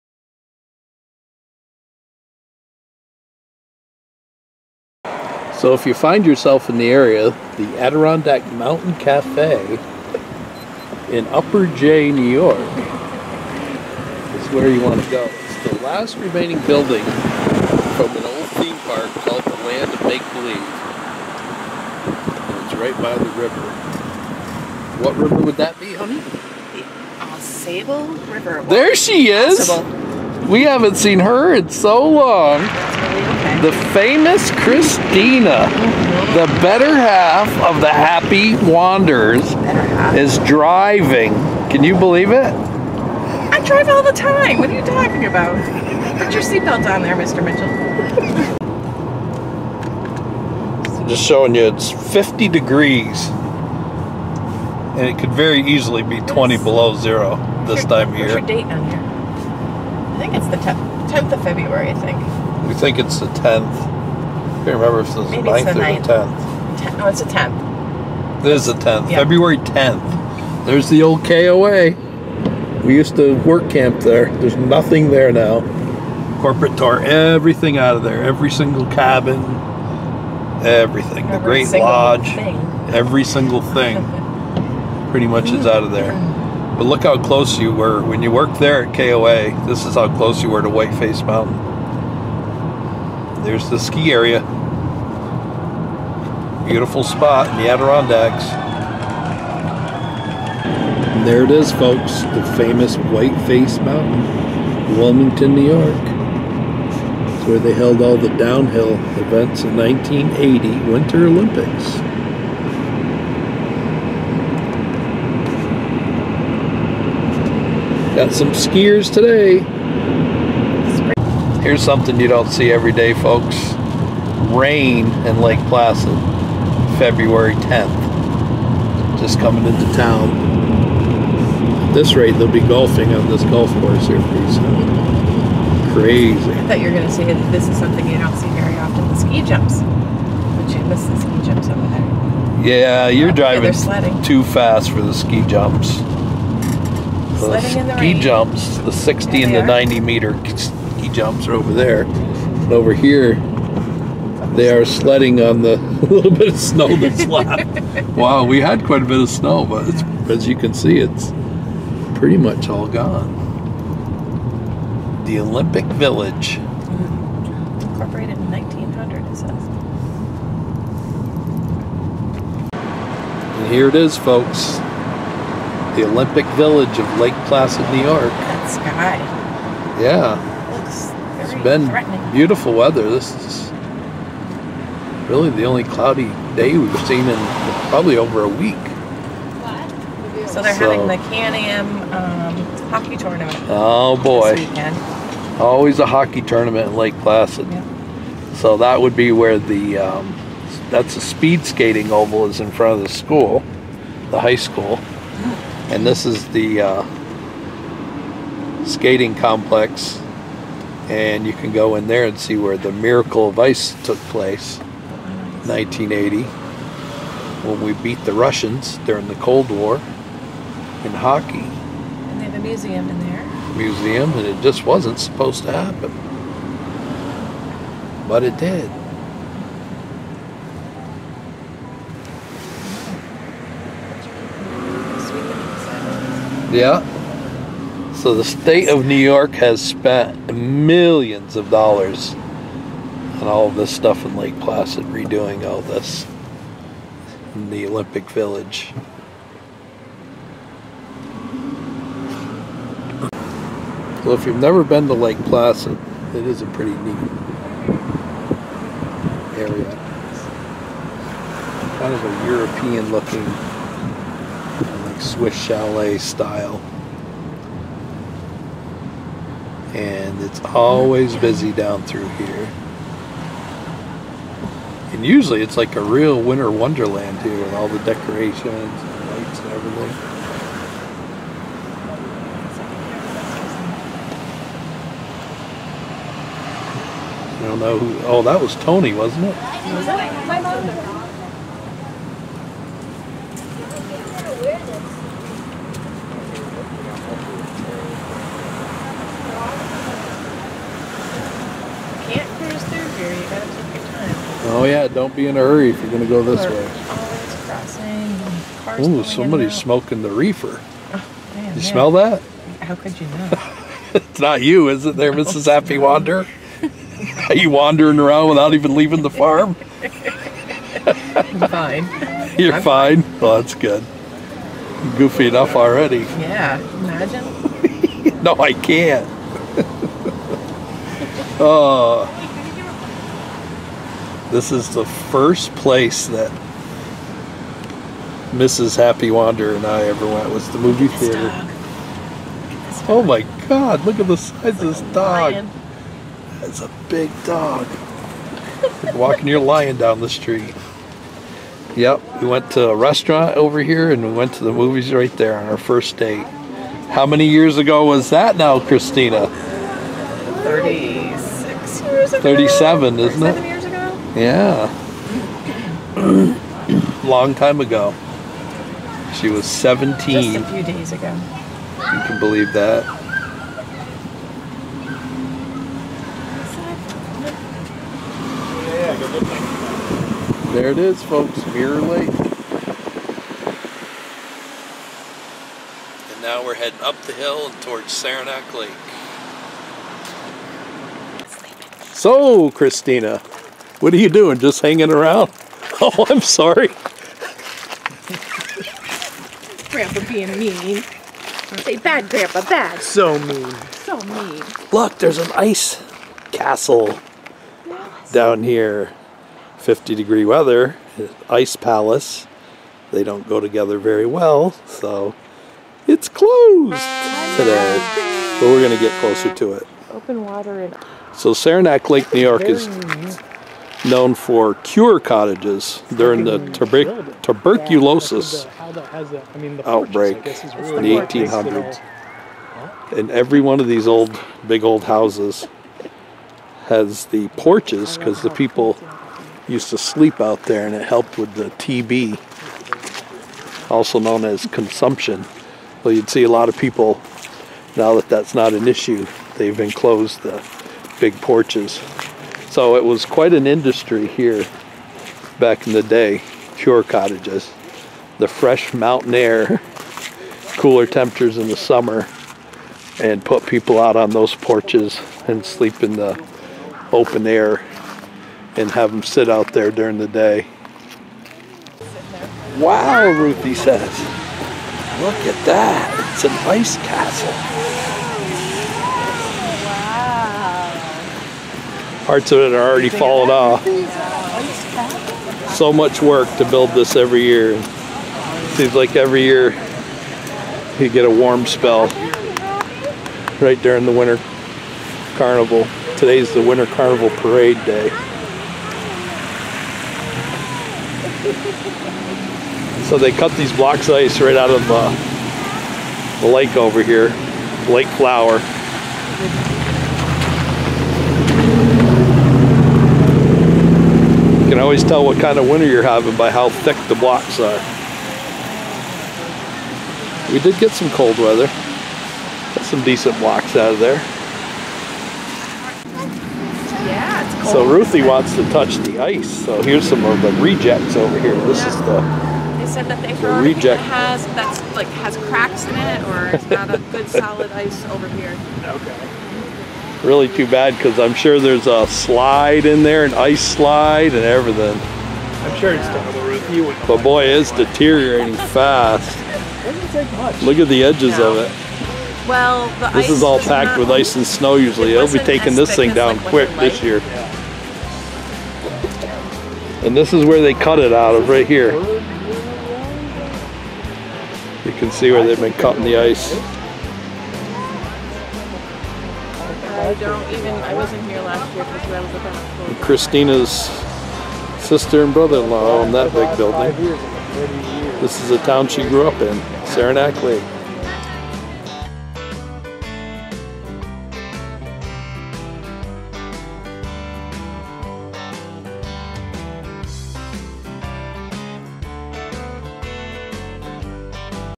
Saranac Lake Ice Palace is HALF MELTED!
WATCH THIS ONE, NO WATER MARK!
We bring you through Lake Placid, NY and into Saranac Lake NY, where we saw the half melted Ice Palace there.